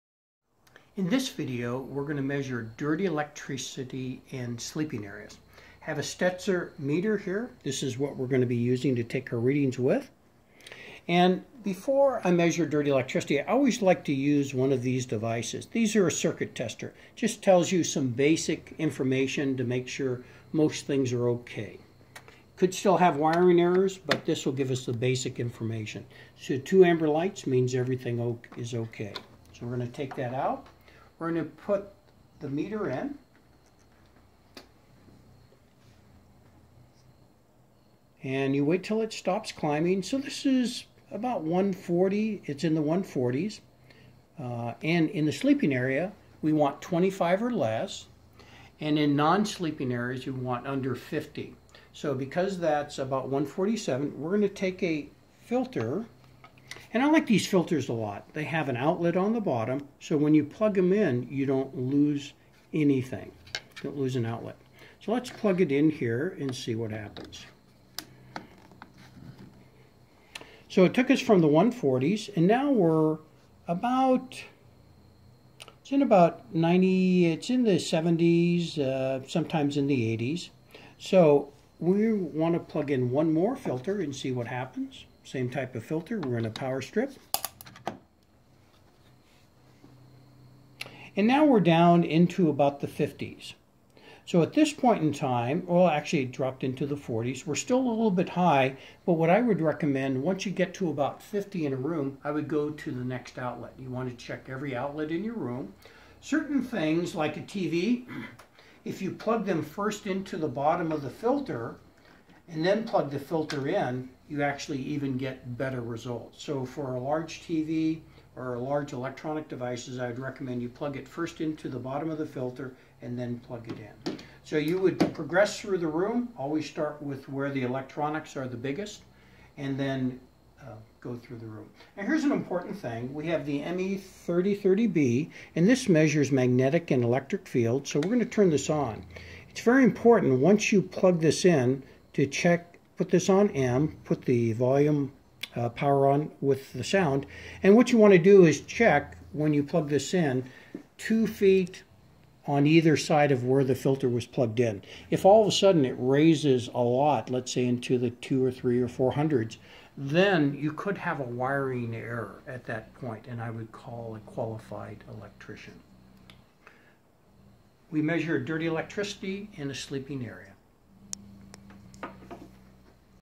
Sleep Hygiene- Cleaning Your Bedroom of Dirty Electricity
In this video we measure dirty electricity in sleeping areas using a Stetzer meter, and how to clean it up using Greenwave Filters. The Greenwave filters are so easy to use and immediate reduces the dirty electricity.